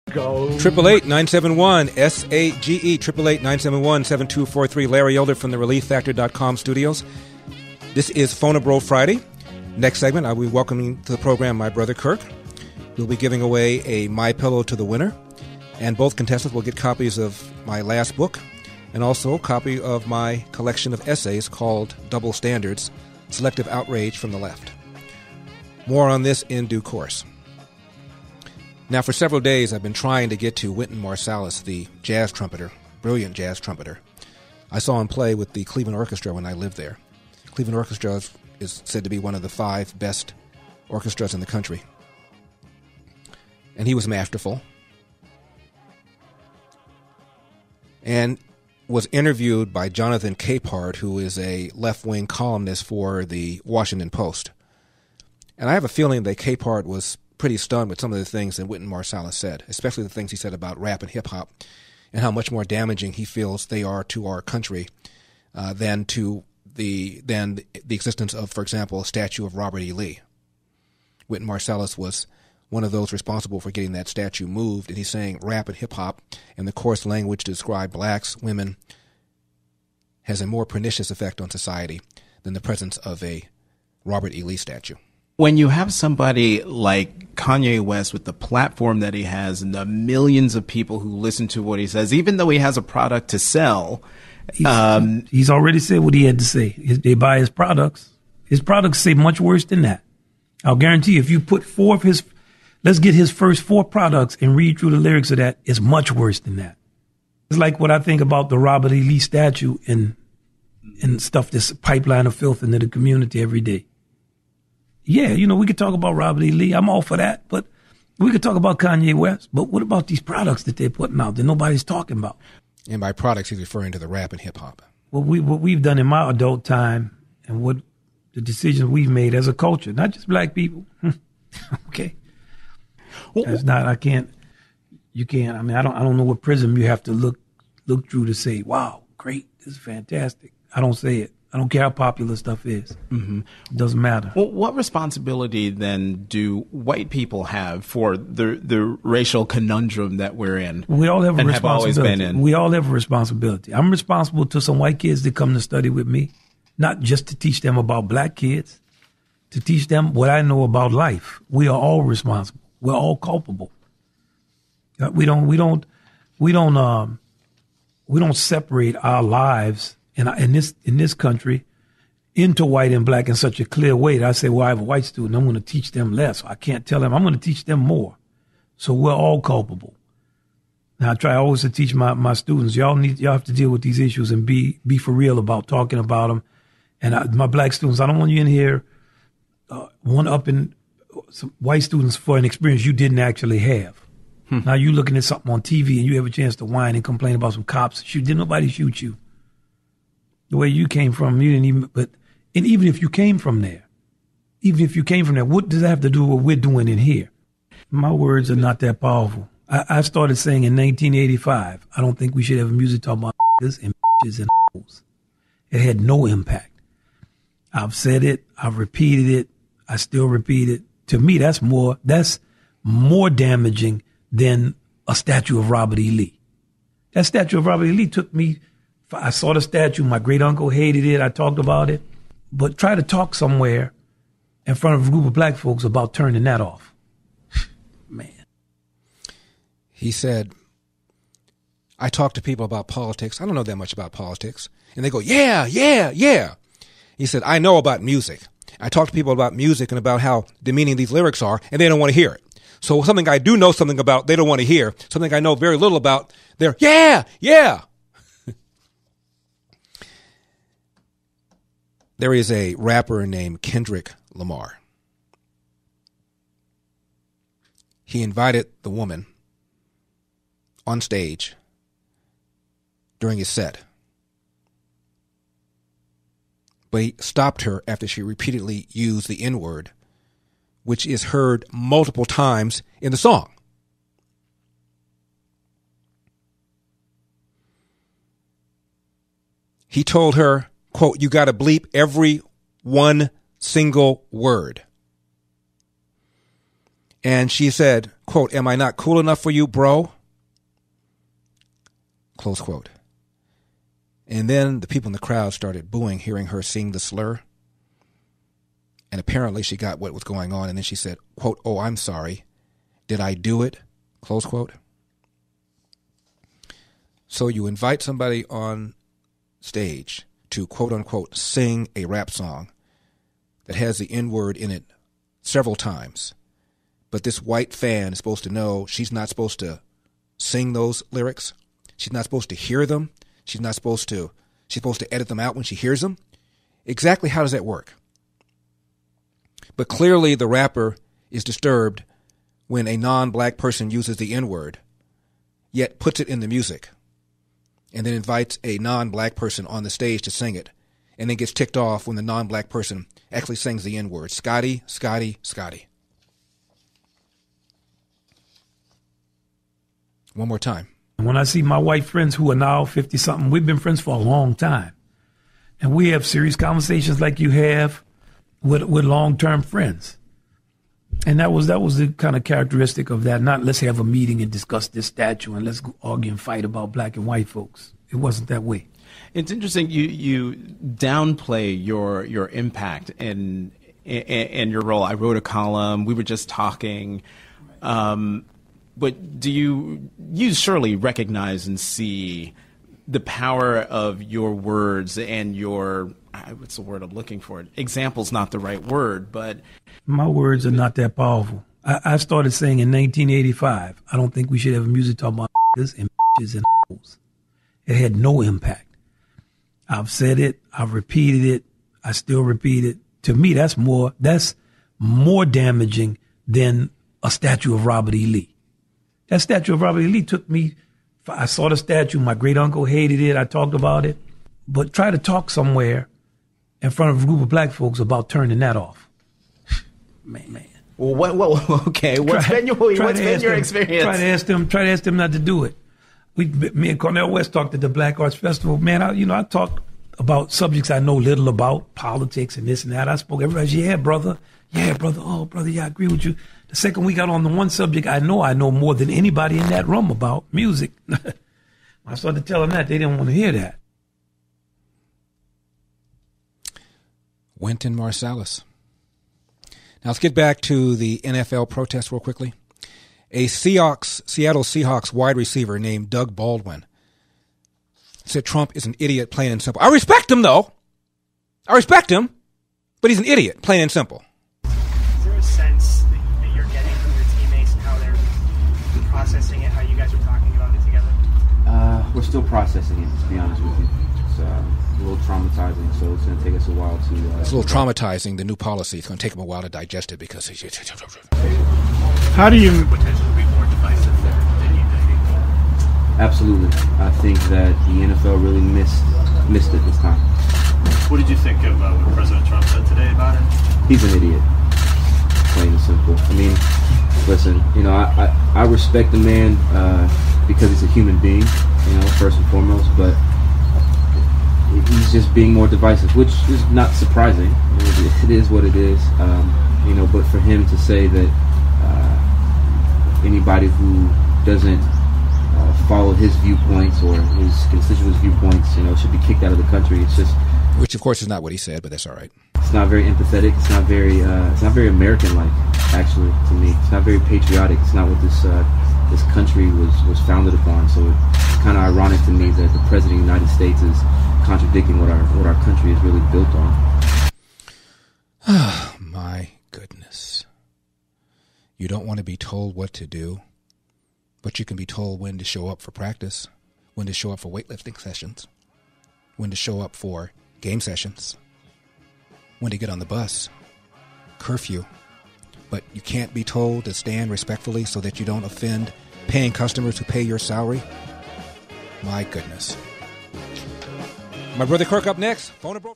Larry Elder - Wynton Marsalis SLAMS Rap Music | Kendrick Lamar is a HYPOCRITE About The N-Word
Check out Larry Elder "Stupid Black Men: How to Play the Race Card--and Lose"